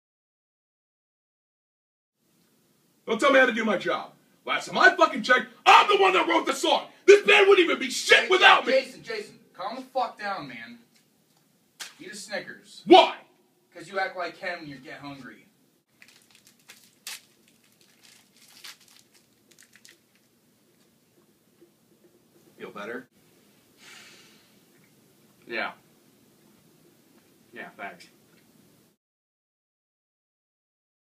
Jason from Fall of Olympus needs a Snickers
Jason from Fall of Olympus gets a little angry when he's hungry